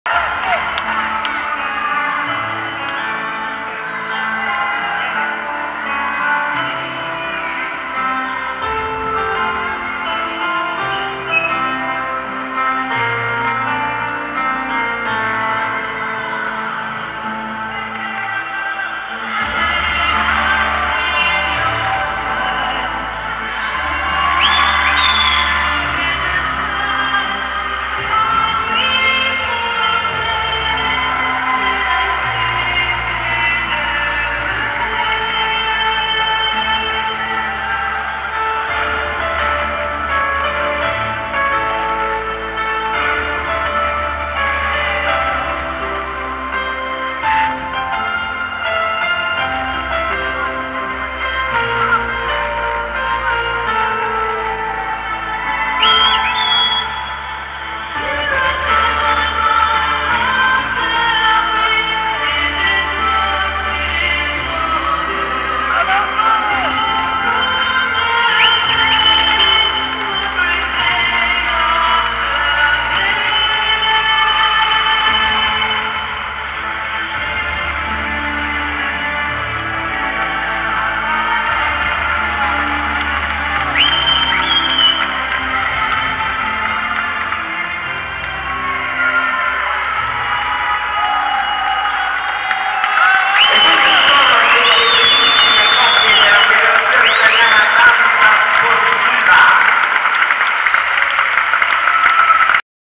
Assoluti Fids 2017 | Finale 16/18 | Valzer Lento
1.Kristina CHARITONOVAITE / Marco BODINI
2.Adele DIODATI / Luciano DONATO
3.Martina ROSSI / Matteo MILINI
4.Vanessa MARTELLI / Gianluca GHIRELLI
5.Anastasiia MARTYNOVA / Daniel BUONARRIVO
6.Francesca CASALINO / Angelo ANNESE